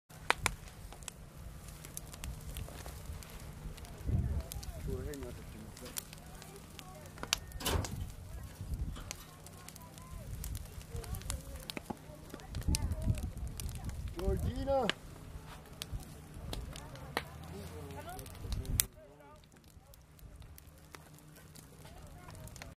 Outside Cooks at Hatchet Lake Denesuline 45th Annual Winter Carnival
Hatchet Lake Denesuline First Nation 45th Annual Winter Carnival
March 4-8, 2015 Wollaston Lake, Saskatchewan